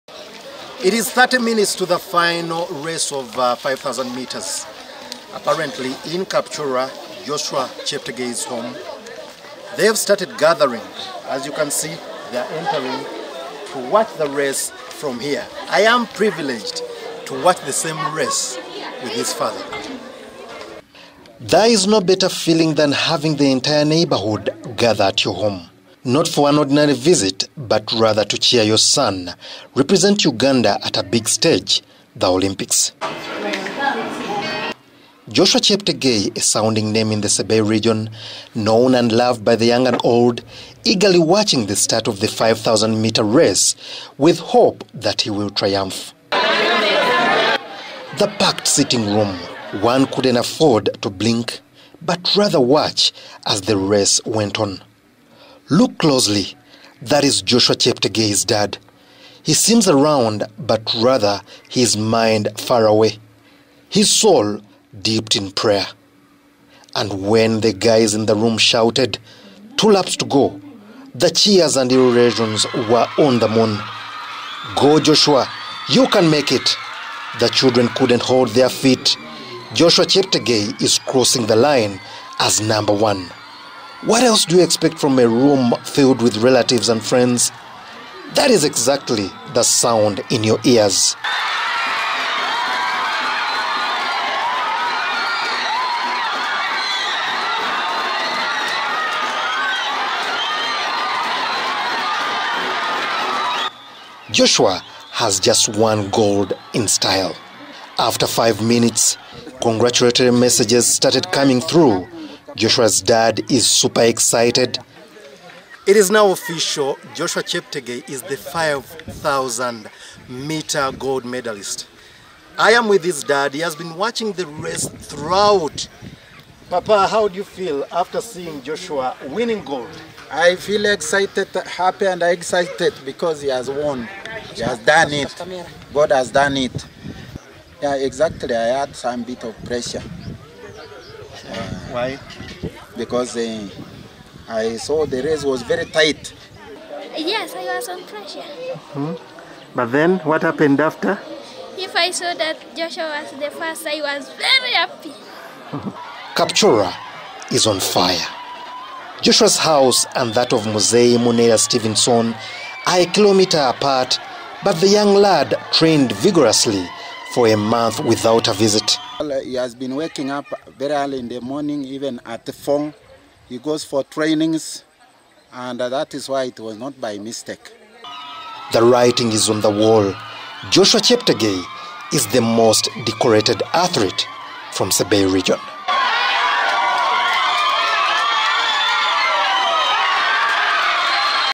Cheptegei celebrated in Kapchworwa - Thousands flock his home after win.
Joshua Cheptegei has won gold in the 5,000 meter race in Tokyo. The gold comes after his success in ten thousand race where he bagged a silver.
The joy in Kapchorwa at Muzee Munerya Stevenson Cheptegeie’s father’s home is earth-shattering as several people flock to the home to congratulate the win.